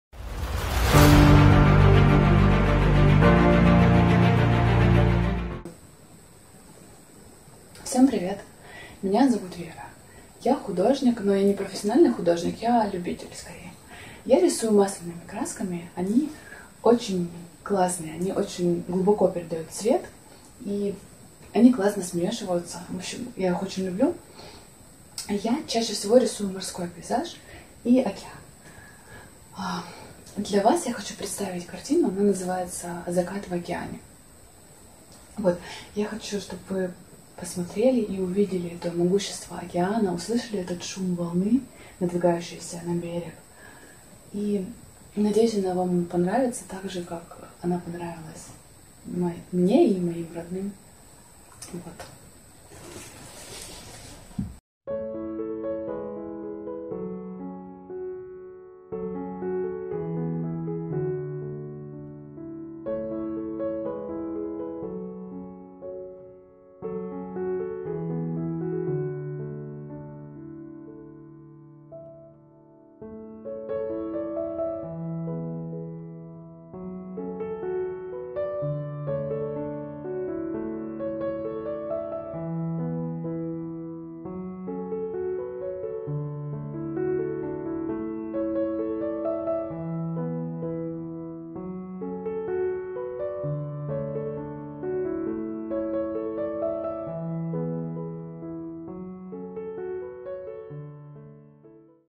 Golden Time Distant Festival | Season 15 | Vera Voskoboynik | GTDU-1501-0054
Имя участника или коллектив: Vera Voskoboynik

Страна: Russia

Название конкурсной работы: Закат в океане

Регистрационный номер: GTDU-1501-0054

Номинация: Живопись